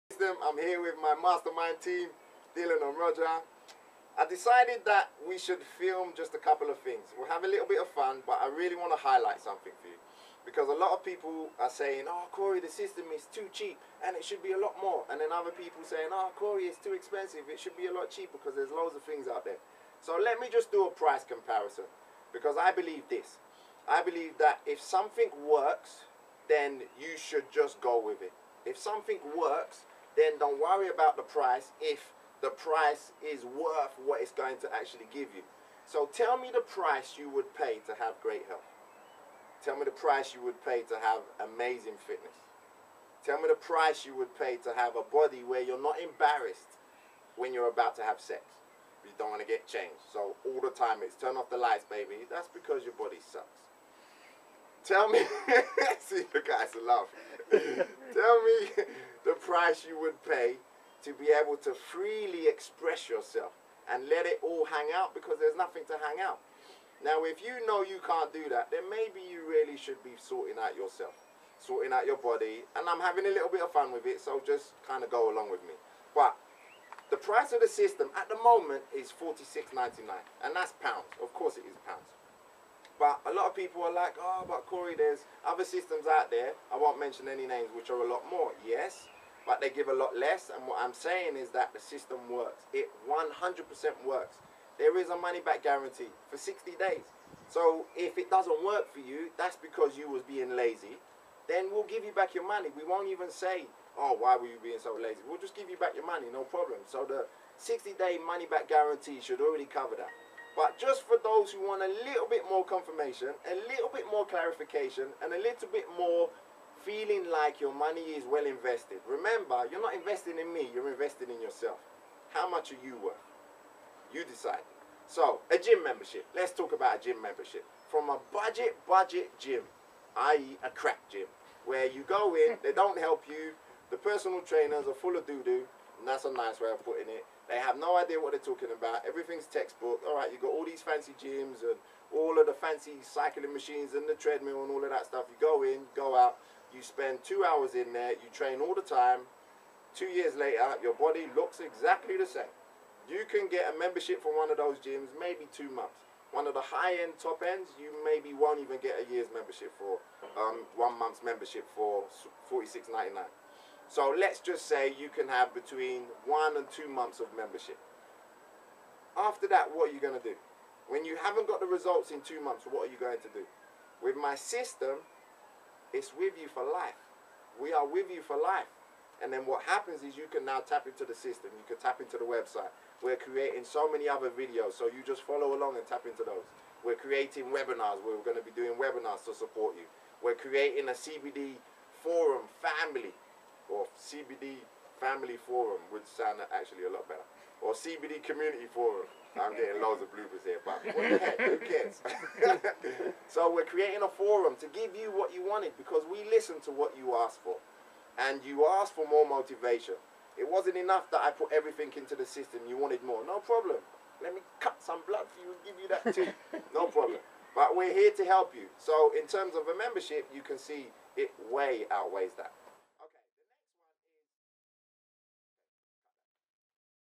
Cbd System cost
Corey has fun while asking you the following serious questions:

What price would you pay for great health? How much would you pay for amazing fitness? What would you pay to have the knowledge and tools to be able to kick fear to the curb, slap up your excuses and destroy procrastination? The key and solution is within the proven CBD System.